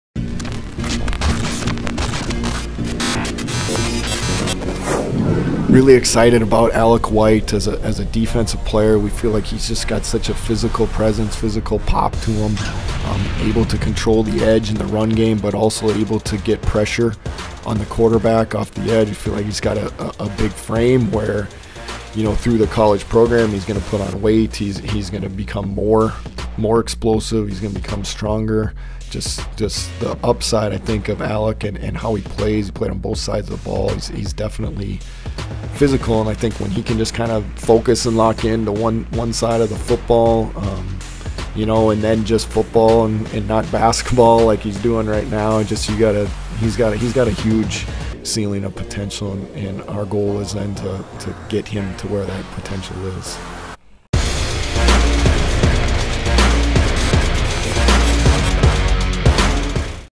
Alec White (DL, 6-3, 220, Wauwatosa, Wis./Wauwatosa West HS)
First-team all-region and first-team all-conference selection…Scholar Athlete…three-year letterwinner in football…also lettered three years in basketball.

“He has a physical presence and pop to him that allows him to set the edge and show off his huge ceiling of potential” - @CoachLaqua